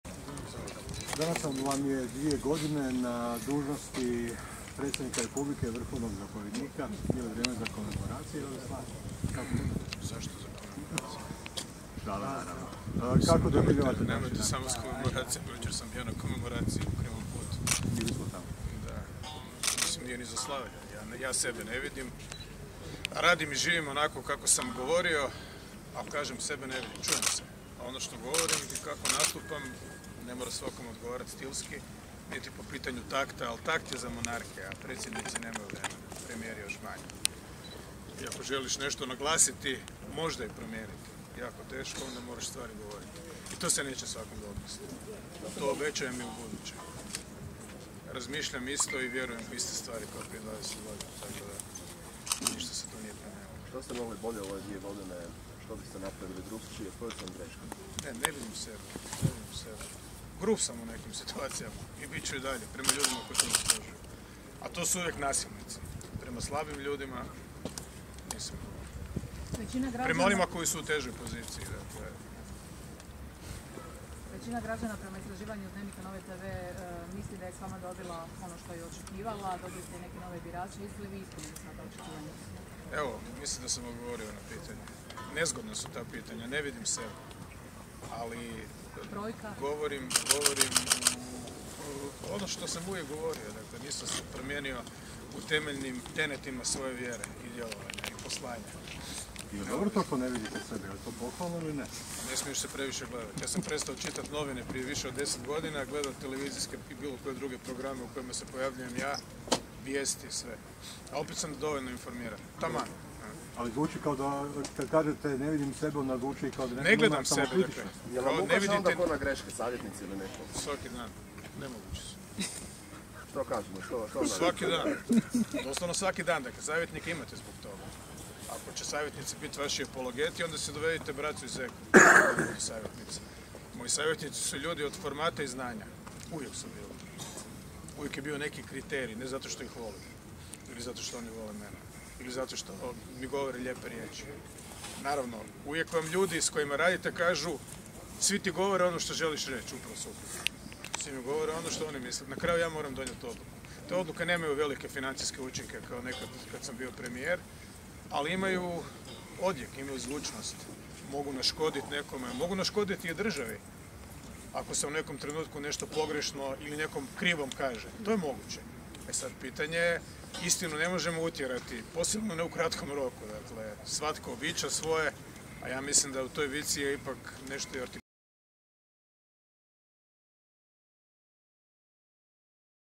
Milanović: Grub sam u nekin situacijama, a to su uvijek nasilnici
Predsjednik Zoran Milanović prisustvovao je svečanosti polaganja prisege 33. naraštaja vojnih ročnika. Nakon svečanosti komentirao je dvije godine svog predsjedničkog mandata.